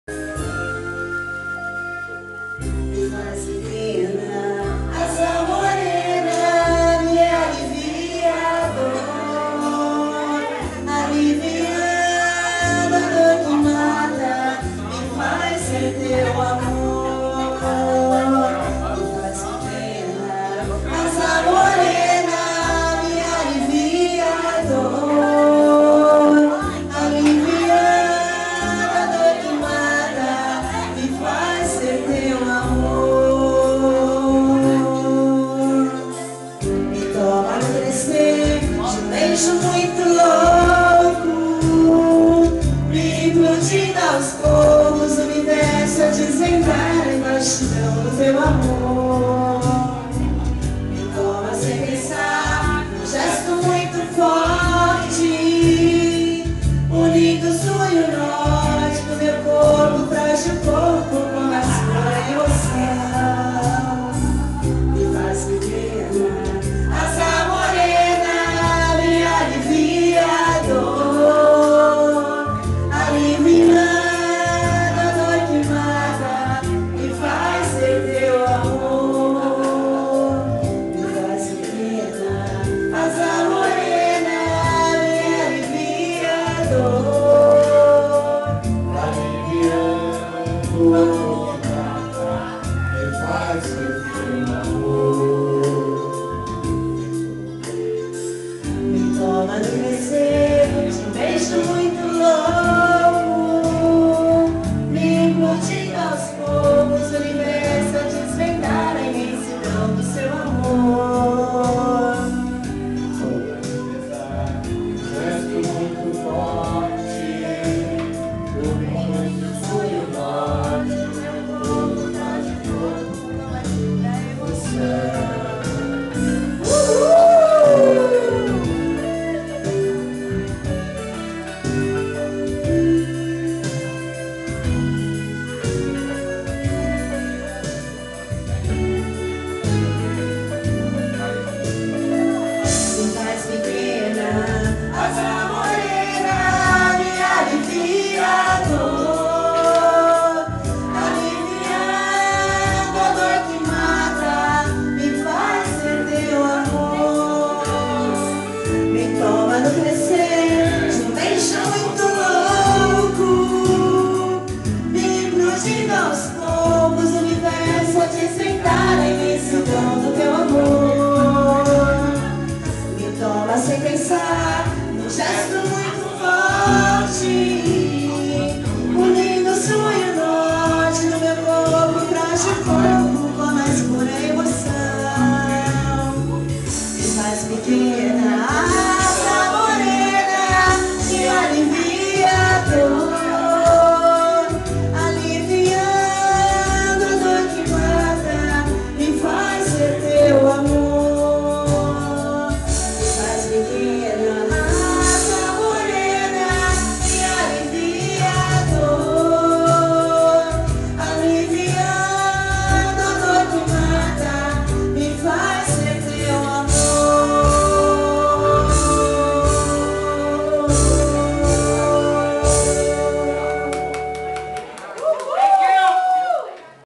KARAOKE
GI FELIX CANTANDO PRO TONINHO E ELE CANTOU JUNTO